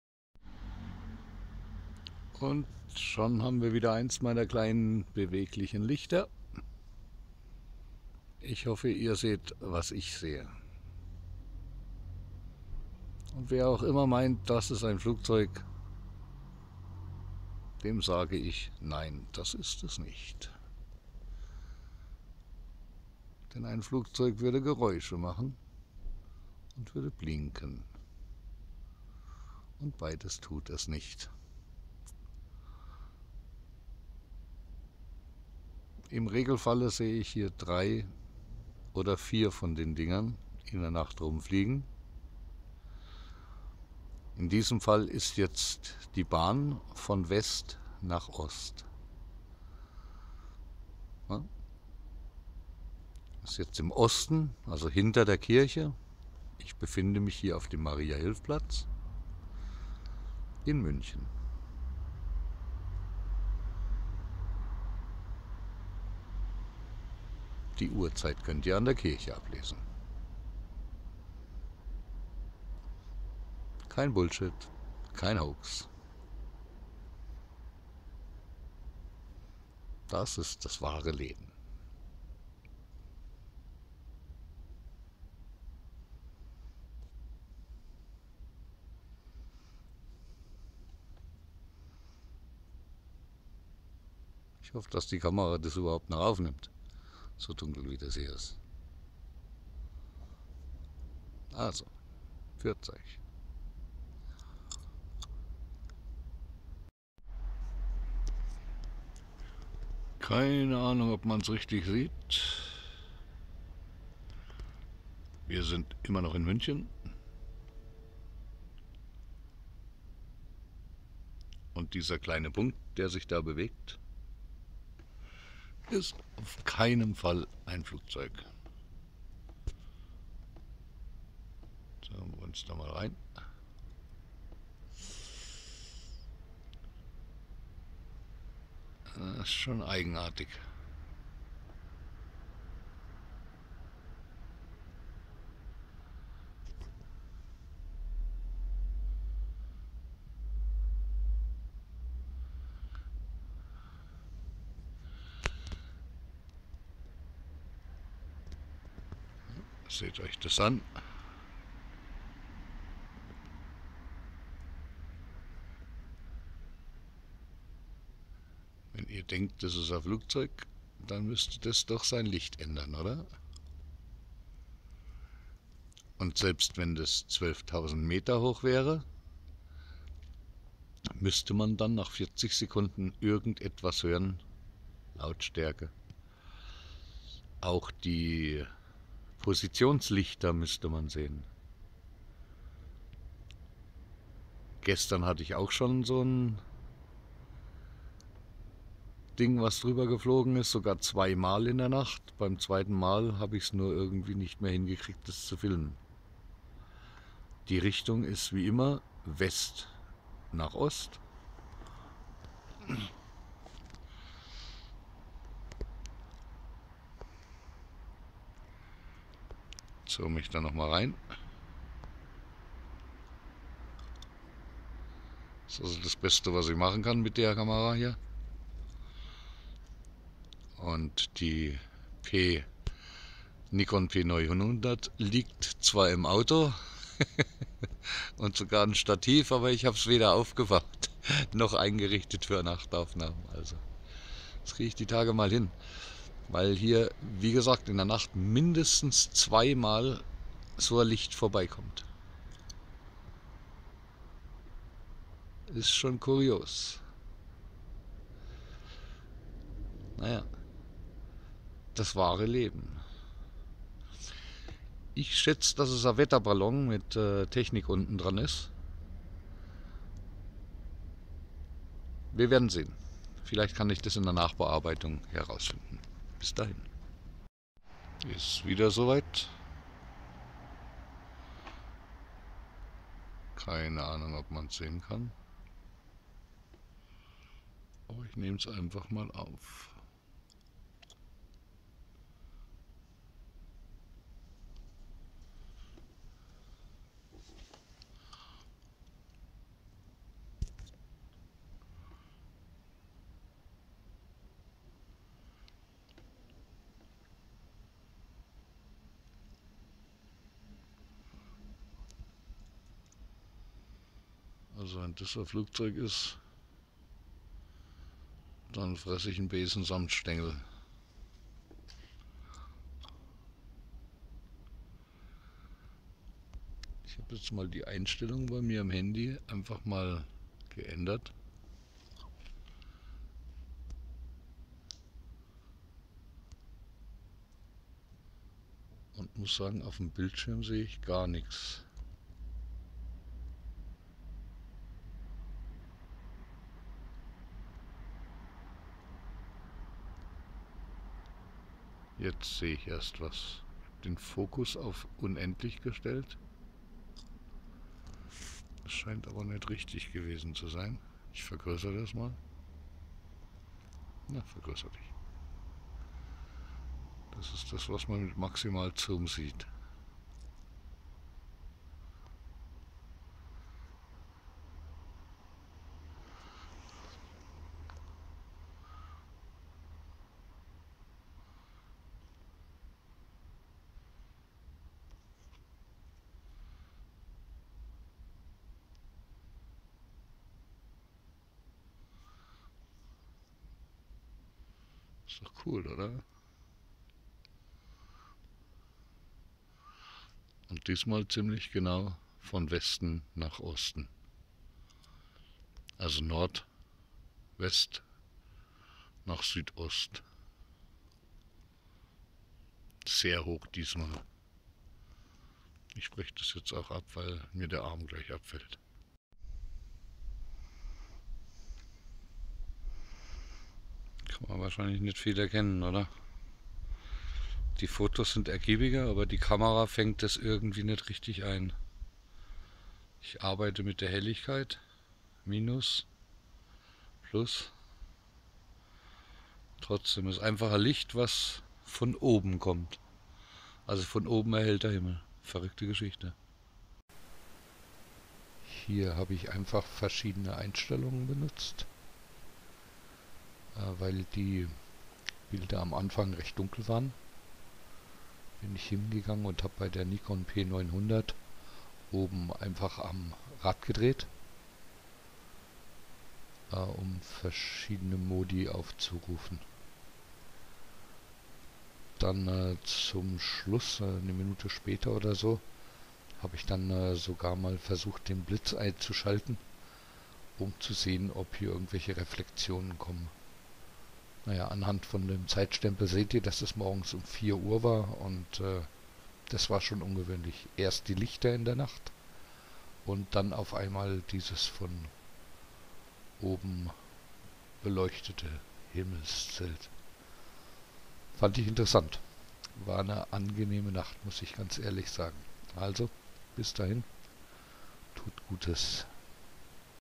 Flache Erde Wahrheit. Lichter über München Originalvideo & Bilder von Lichtscheibe 04:00-04:50 Uhr
Unglaubliches am Münchner Stadthimmel am 26. & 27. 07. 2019. Erst Mehrere Lichter die von WestNordWest, nach OstSüdOst zogen, dann plötzlich am morgen gegen vier Uhr eine helle Lichtscheibe im Norden.
Ja leck hob I mir denkt, des muaß I aufnehmma. Vui Spoß beim zuaschaugn! ;)